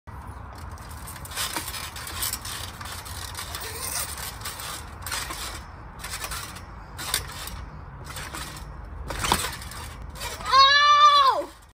BreeLiving Her best life jumping on the trampoline: warning gruesome audio
Breee living her best life, jumping on the trampoline: warning ⚠️ Gruesome   audio ⚠️ Also I am literally in pain, but healing fast 💯

I told Myself I wasn’t going to do a backflip since I never knew how but would try a front flip. So I do it and as I’m trying to land on my bum bounced and actually didn’t, I only landed on my feet WEIRD(😼😺😸).  In the video, you can hear the 💥 pop 💥 sound 💔 sheesh, why I gotta be like this, FR… daredevil for life. I need to take flip lessons and skateboard more or something because I’m always going to want to do this kind of thing 😅🤣😆